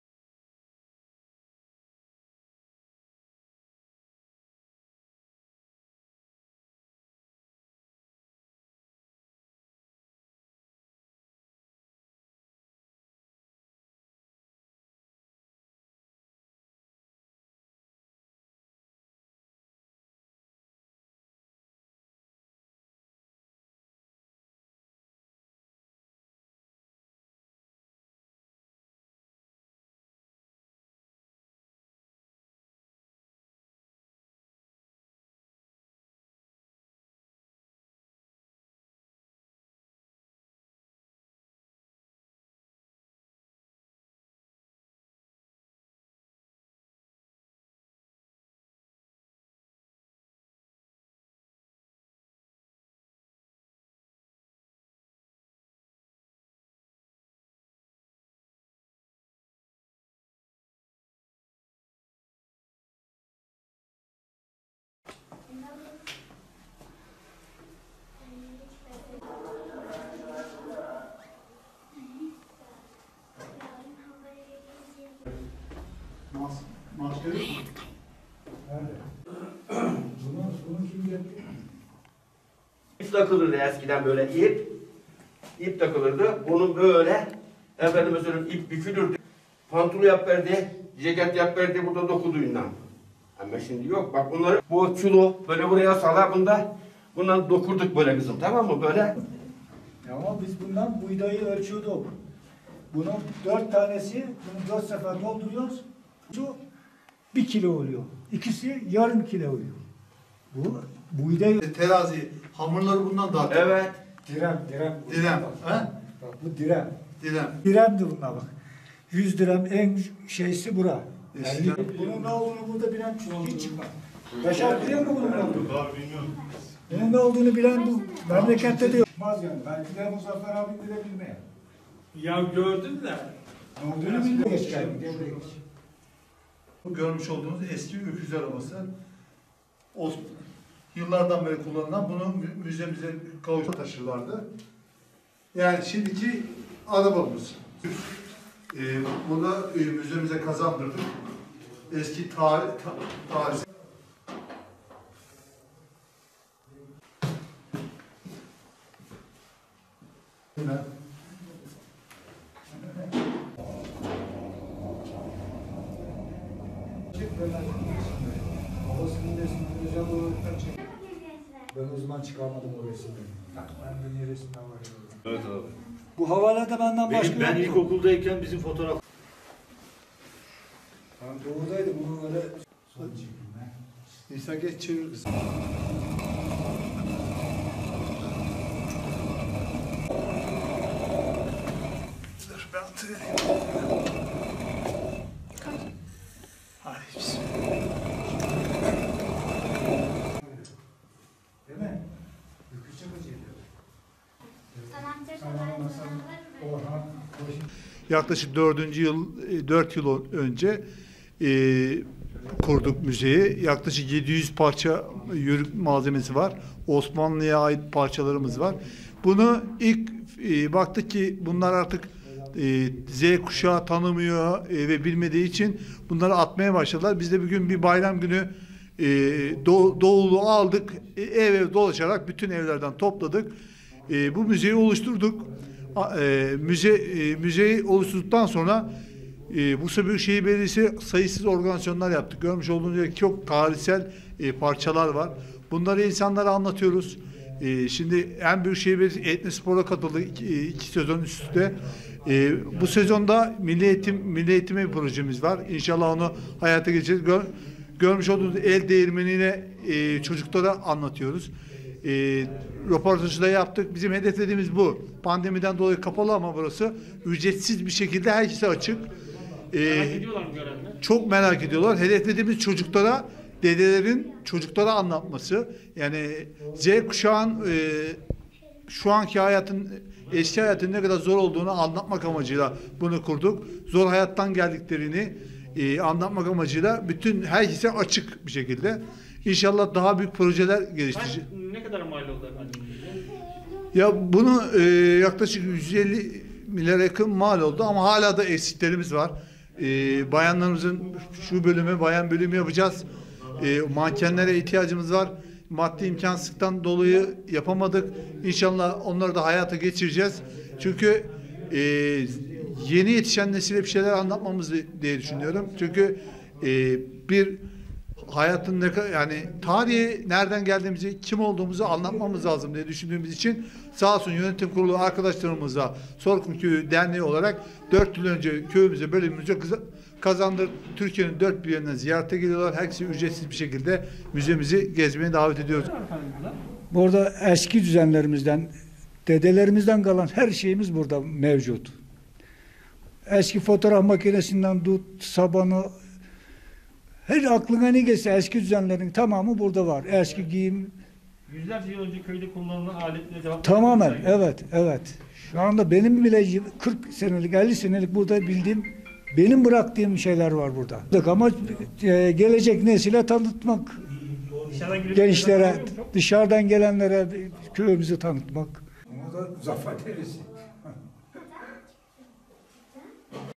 Kendi aralarında para toplayıp köye müze kurdular
Bursa'da köylüler, kendi aralarında para toplayıp, köylerinde içinde yüzlerce ürününü bulunduğu müze açtı.